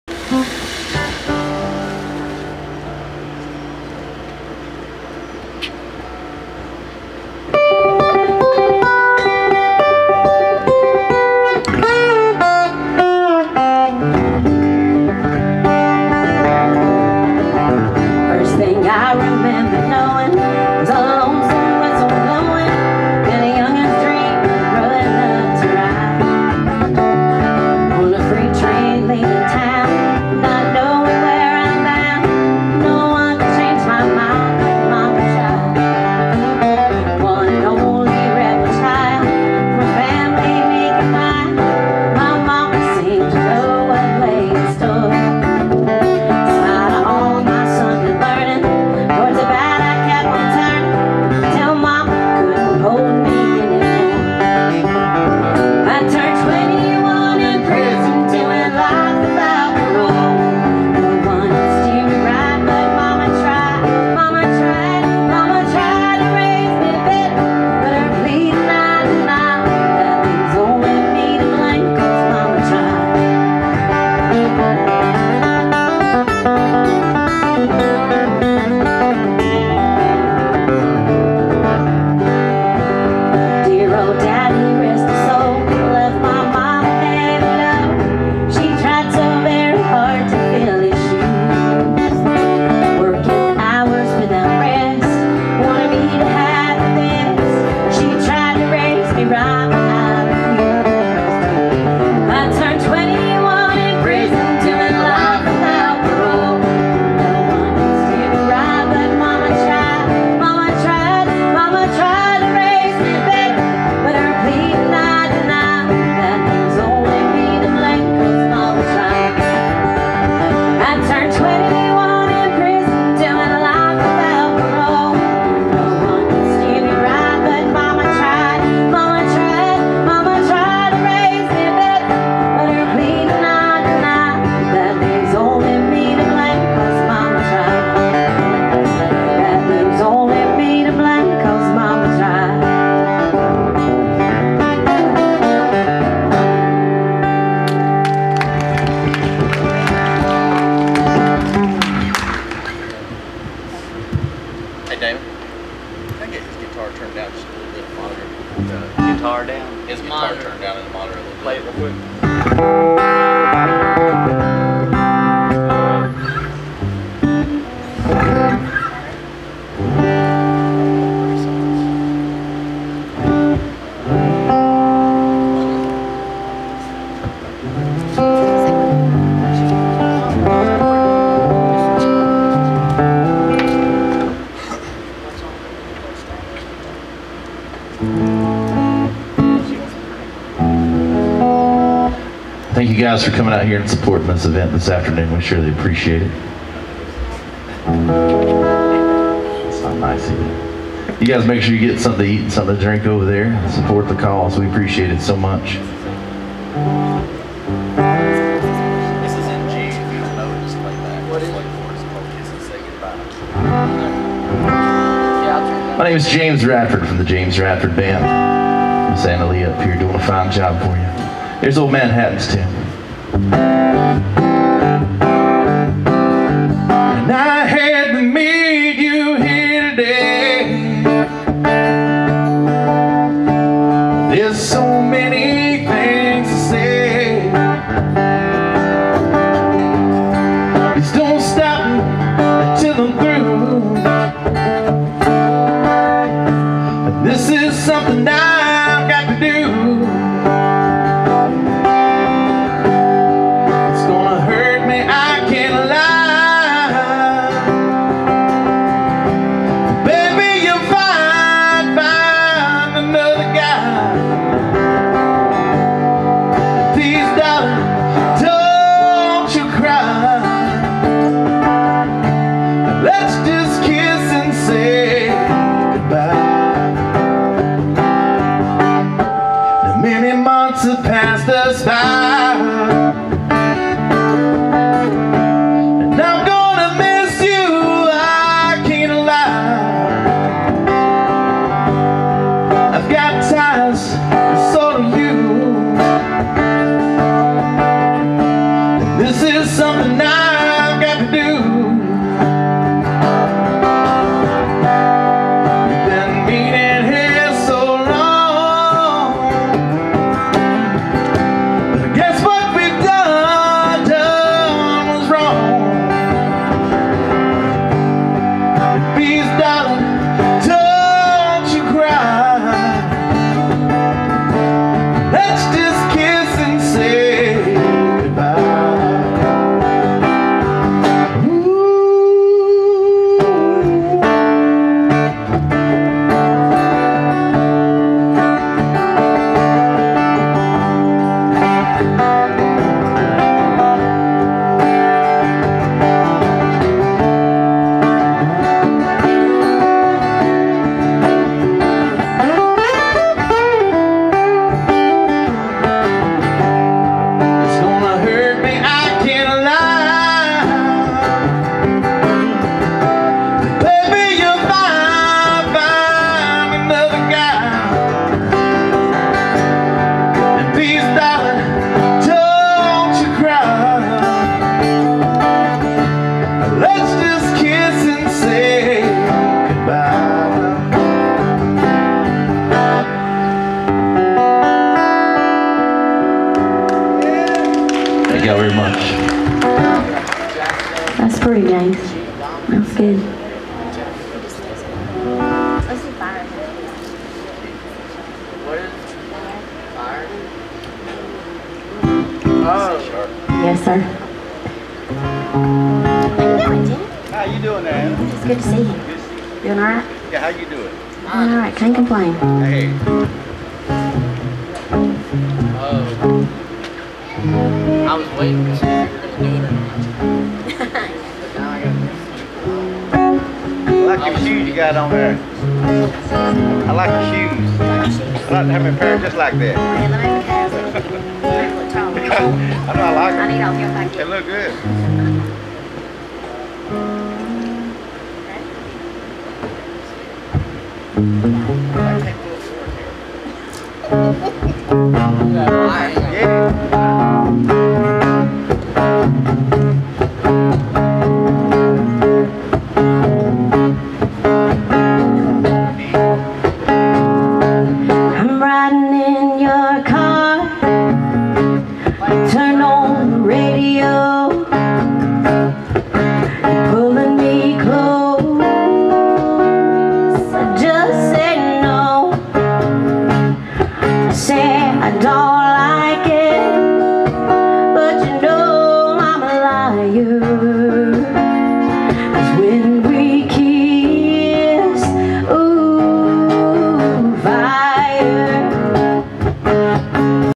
Anna Leigh & James Radford perform at Carolina Wren Park..10-12-2017
Concert for Hurricane,Direct Relief.
David Standard,concert promoter.
Set HQ quality at 1080p for better viewing of video.